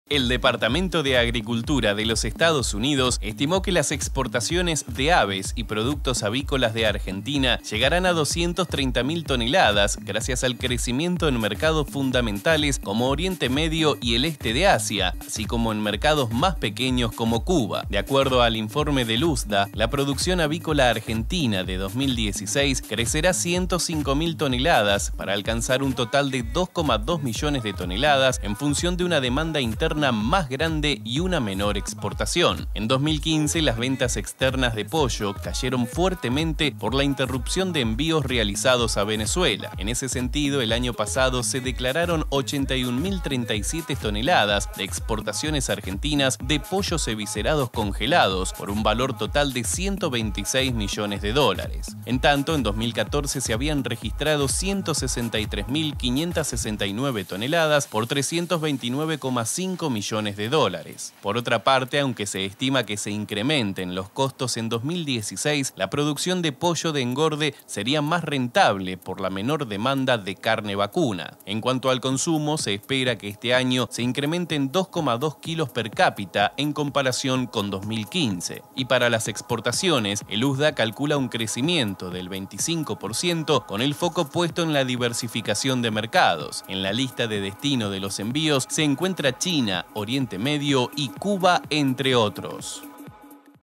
PamperoTV.17-03-2016. USDA estimó que la producción de aves y productos avícolas crecerá este año.
El Departamento de Agricultura de los Estados Unidos, estimó que la producción de aves y productos avícolas crecerá fuertemente este año gracias a la diversificación de mercados y el menor consumo de carne vacuna.